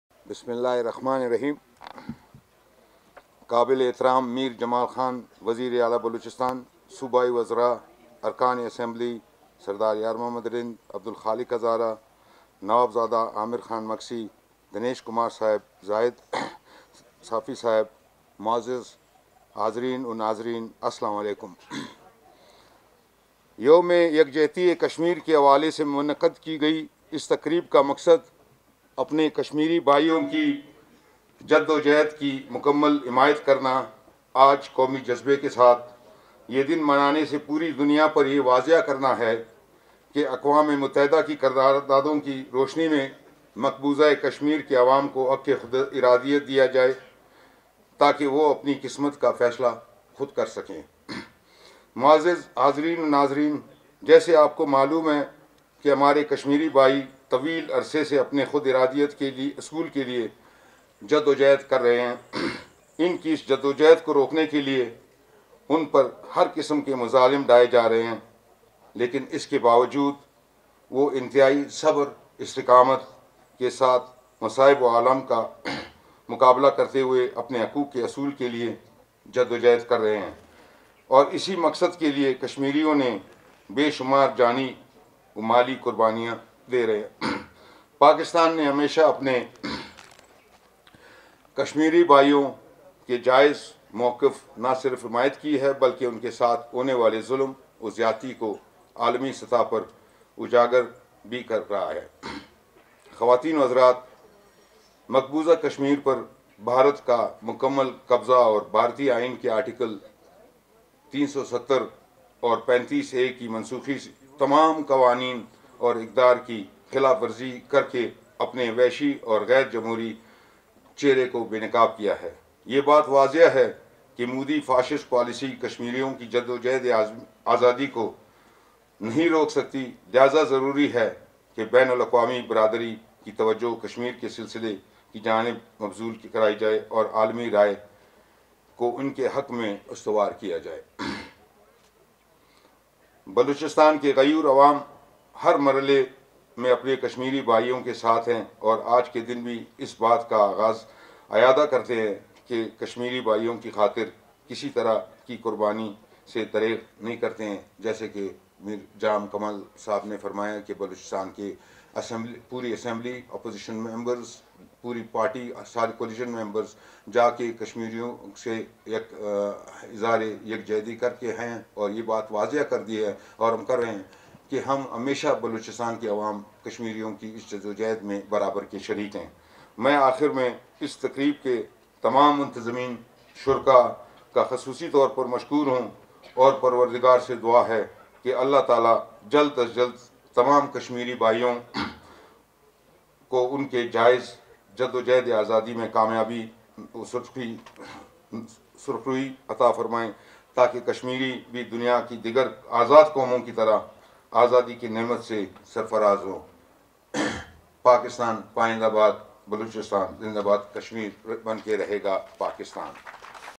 Governor Balochistan Amanullah Khan Yasinzai Speech about Kashmir solidarity day
Kashmir solidarity day program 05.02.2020